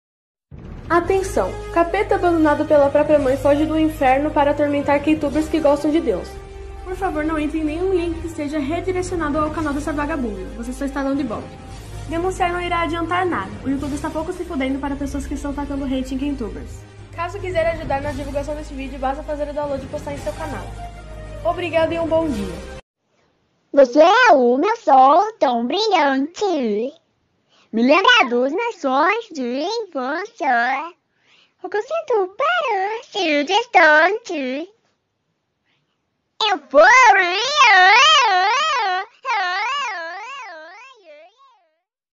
Tem uma racista do kpopper chamada kim maria hyung,entao eu postei esse recado pra ela + aviso desc
Veio pelo GC (Gabriel Cardoso)?
Meu canal ta cheio de inscrito fantasmas oh seja não acompanham
Por isso pedi ajuda a ele..

Muito obrigado